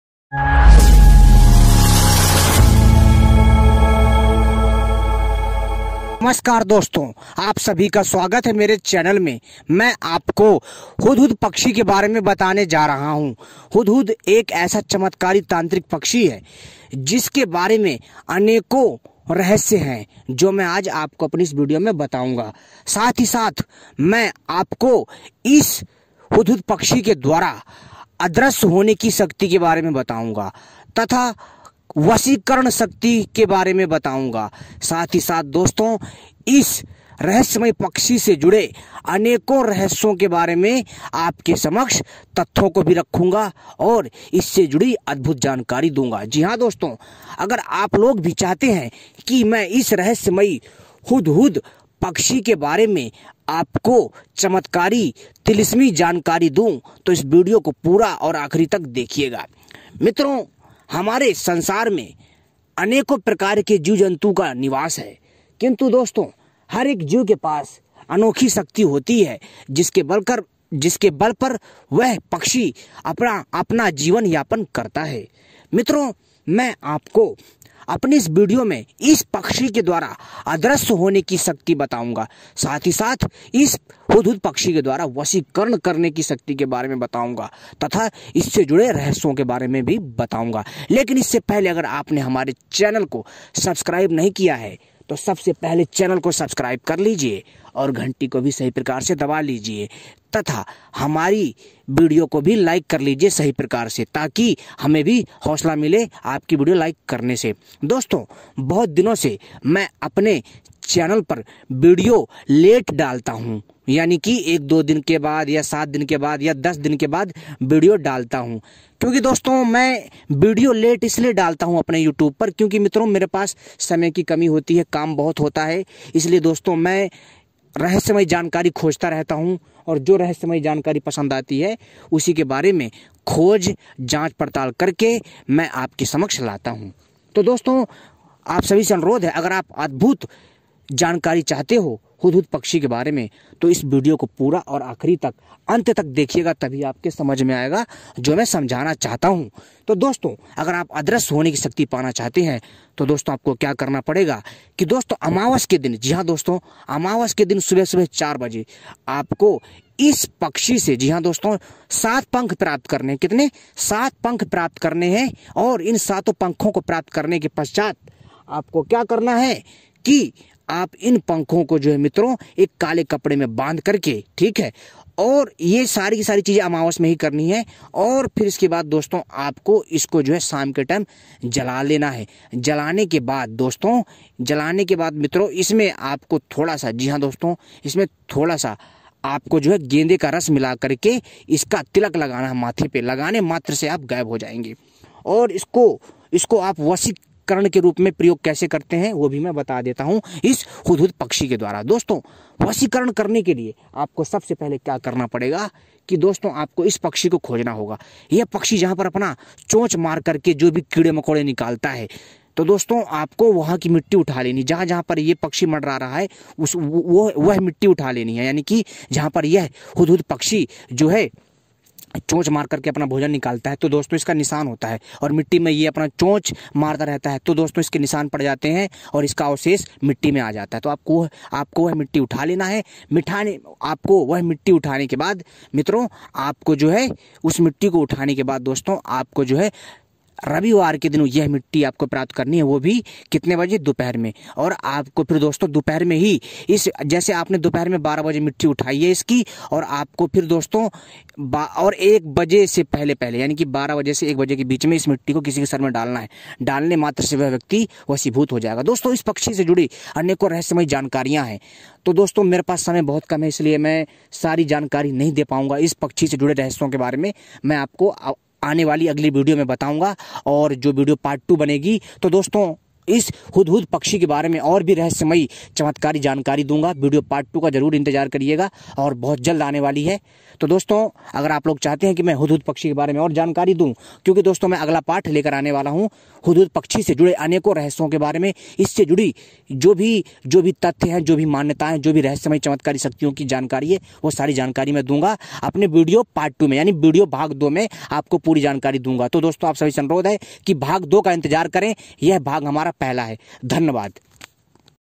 इसके 7 जादुई पंख आपको अदृश्य होने की तांत्रिक शक्ति दे सकता है,जानने के लिए देखें पूरा video//
इसके 7 जादुई पंख आपको गायब यानी अदृश्य होने की तांत्रिक शक्ति दे सकता है,जानने के लिए देखें video//
# hudhud,hudhud pakshi ki janakri,hudhud ke tantrik fayde,hudhud pakshi se paye adrushya hone ki sakti